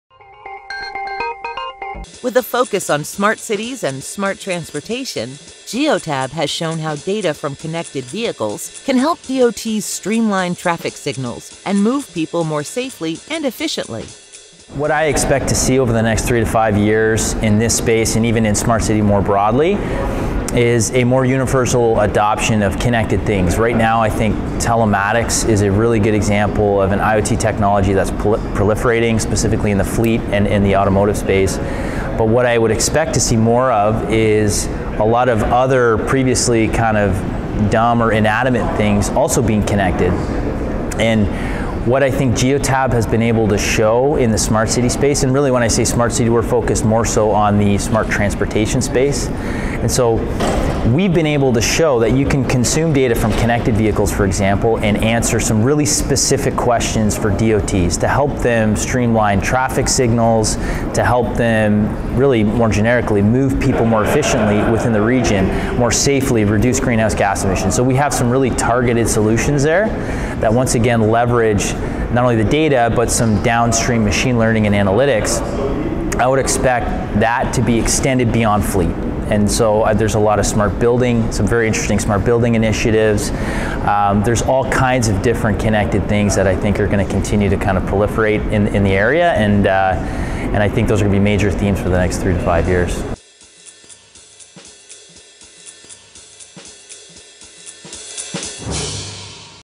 The Right Data Enhances Smart Cities and Smart Transportation | Fleet Management Weekly
With a focus on Smart Cities and Smart Transportation, Geotab has shown how data from connected vehicles can help DOTs streamline traffic signals and move people more safely and efficiently.

JEAN PILON-BIGNELL
VP of Business Development
Government and Smart Cities